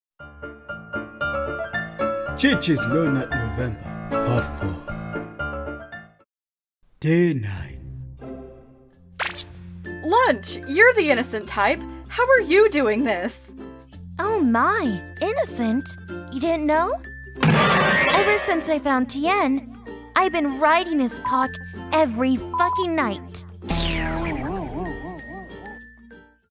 Chichi's NNN Part 4 | DBZ Comic Dub
Chichi is taking the No Nut November Challenge!
Can she make through?

This comic was made by the very talent Funsexydb

Voice Of Lunch: cCuntu

What did you all think of Chichi's NNN Part 3 | DBZ Comic Dub before watching this video?

And would you wanna see more videos like Chichi's NNN Part 4 | DBZ Comic Dub